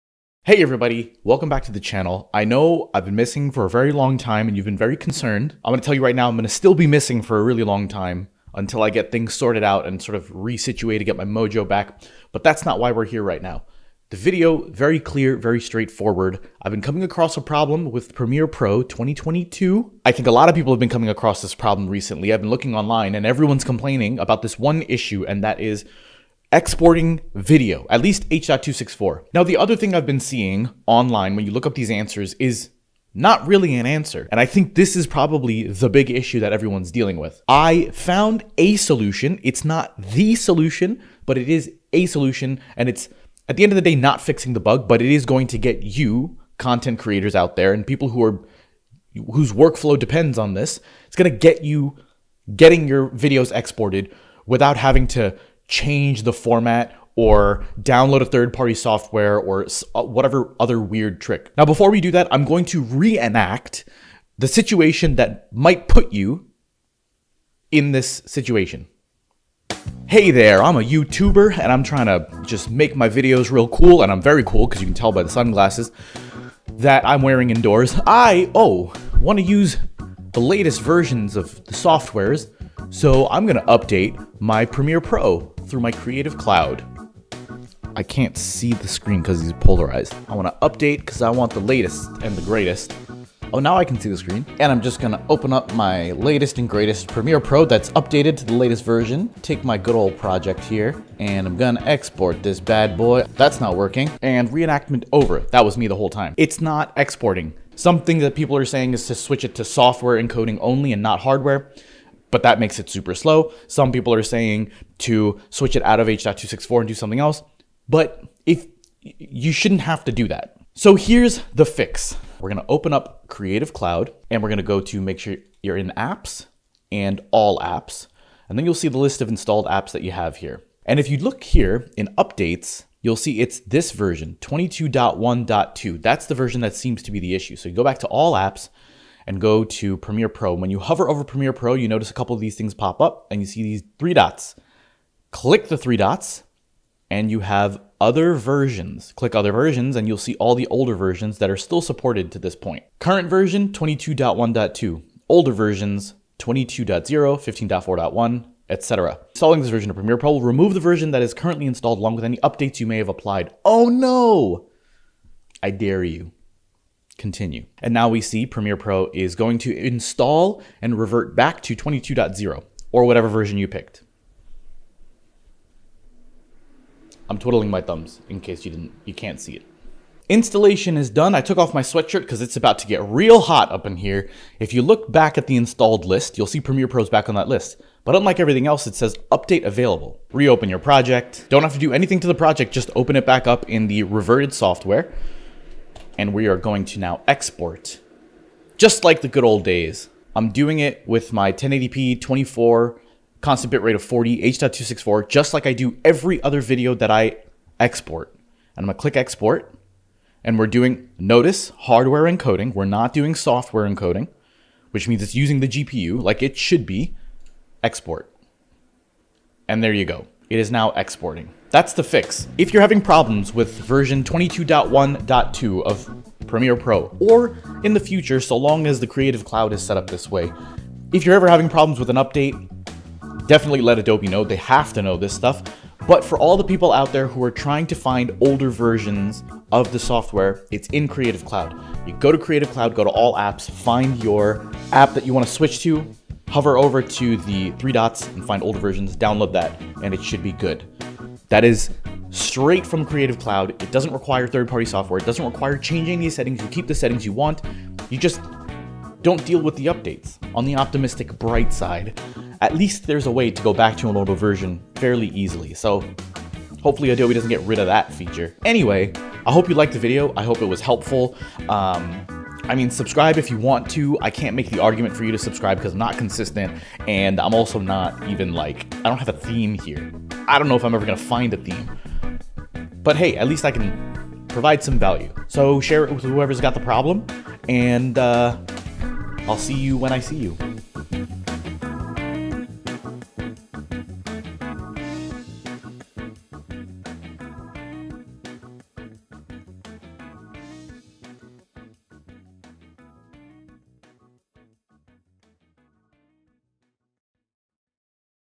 How to REVERT to older Premiere Pro versions (without third-party software)
Have you been experiencing this nonsense with Premiere Pro 2022 where you can't render your H.264 file?

Well FORGET any other solution, and just follow this tip!

00:00 - Intro
01:07 - Dramatic Re-enactment
01:38 - The Problem
02:00 - The FIX
03:42 - The End

I hope this works for you and share your successes down below!